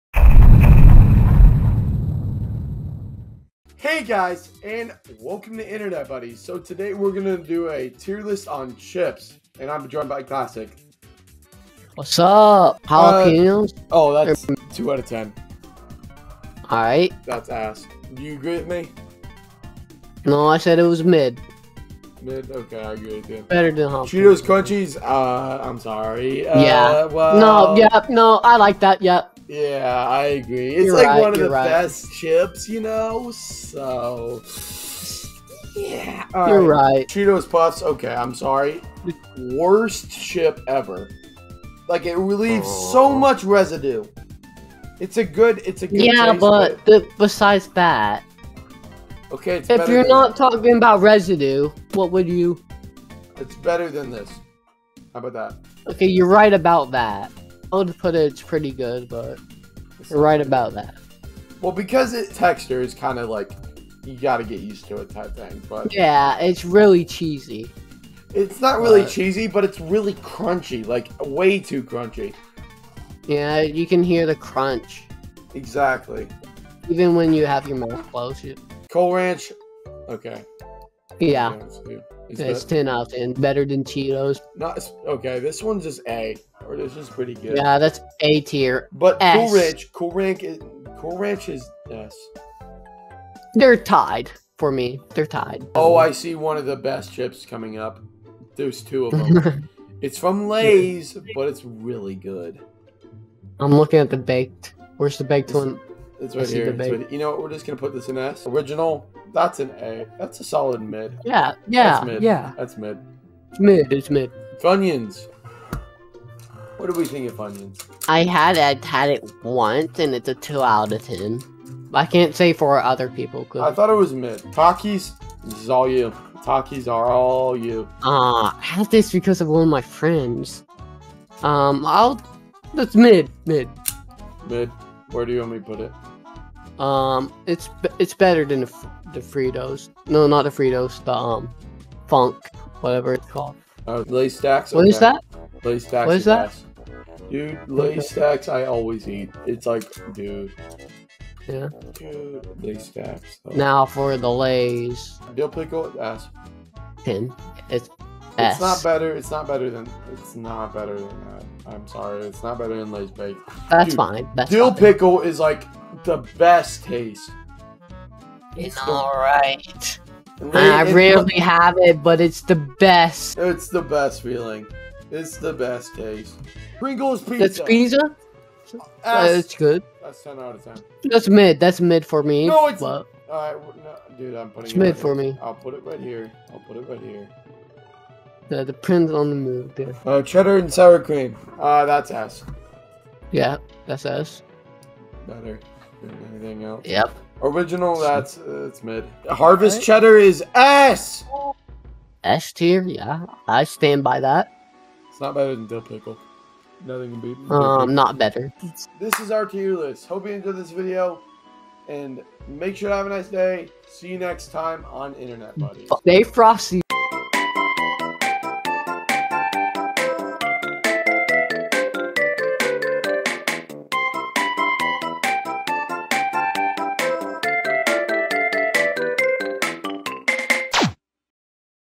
Chip Tier List(Week 9)
Archer and Shane rate chips on a Tier List

Chapters:
0:00 Intro
0:12 Jalapeño Cheetos
0:23 Cheetos Crunchy
0:37 Cheeto’s Puff’s
1:18 Doritos Cool Ranch
1:24 Doritos Nacho Cheese
1:35 Lay’s Baked Original
1:48 Fritios Original
1:54 Funyuns
2:04 Taki’s
2:23 Lays Stax
2:35 Lays Dill Pickle
2:56 Pringles Pizza
3:10 Ruffles Cheddar & Sour Cream
3:17 Ruffles Original
3:19 Sun Chips Harvest Cheddar
3:36 Outro